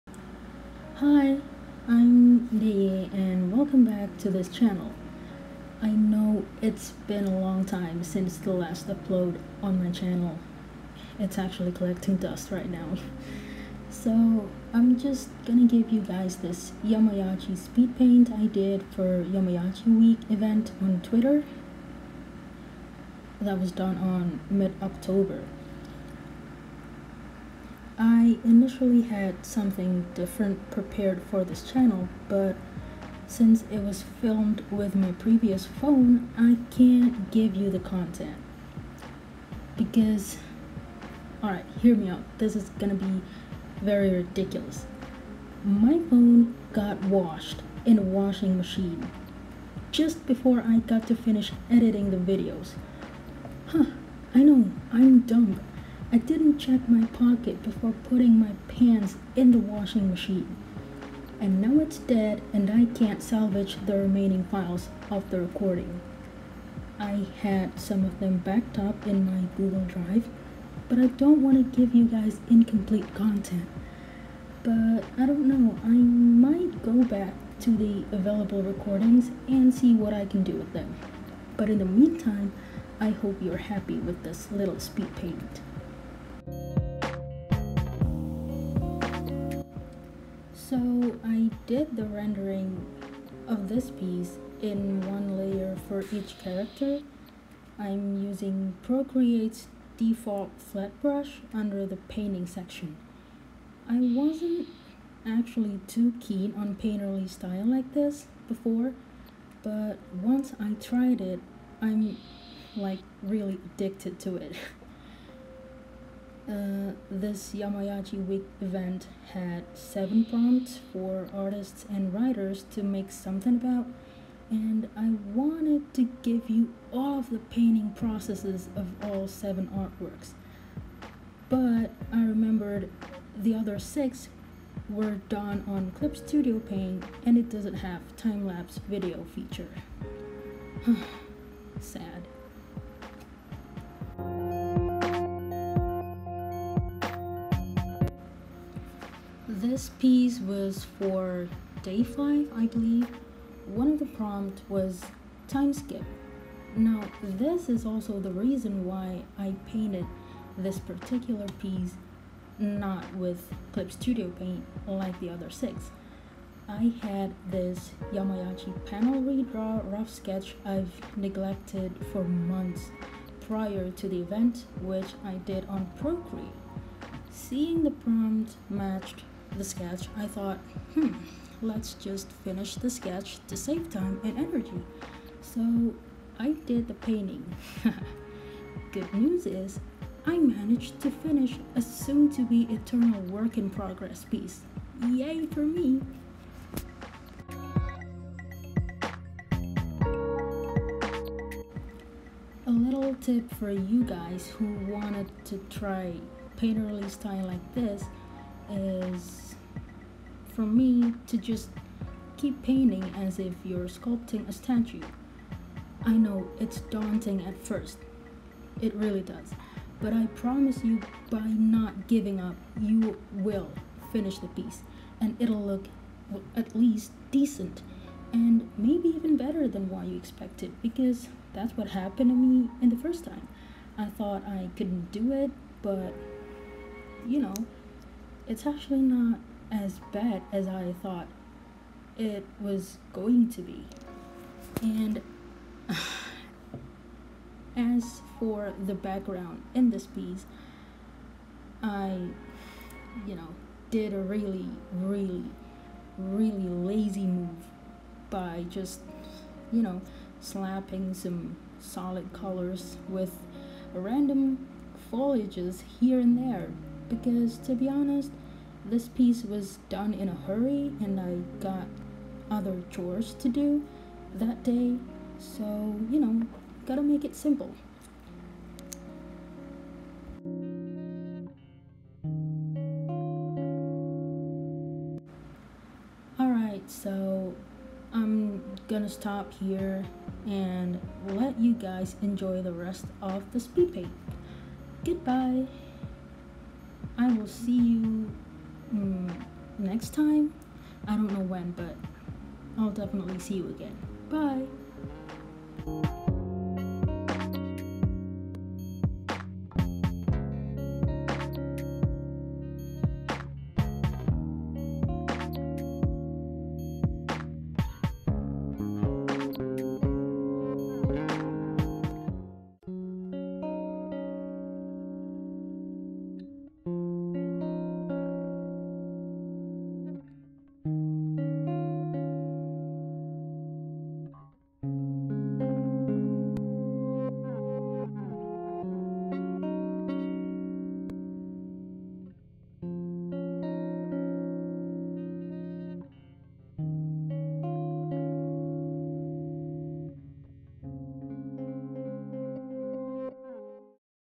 Painting Timeskip Yamaguchi and Yachi!! | Speedpaint + Voiceover | Procreate
Hi! Back again with me on another speedpainting video! Today I’m gonna walk you through my YamaYachi painting I did for a Haikyuu fan event on Twitter back in mid October 2020 called YamaYachi Week hosted by @KazuHattori (twitter), where artists and writers can participate by submitting their works following the pre-determined prompts (or you can just stray from them and let your imaginations run wild) for seven days consecutively.

FOLLOW ME ON OTHER SOCIAL MEDIA!

SOFTWARE USED
- Procreate (drawing and speedpaint recorder